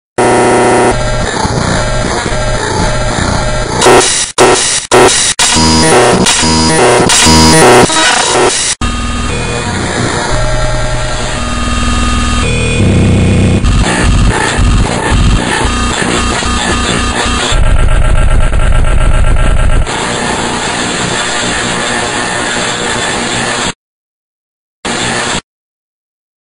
(Reupload) The hemispectrum and malware is attacking the newer spectrums
Round 7 for the custom effectless WMM tennis match made by Massivezephyr14. He recently closed his account.

Originally released: November 14, 2010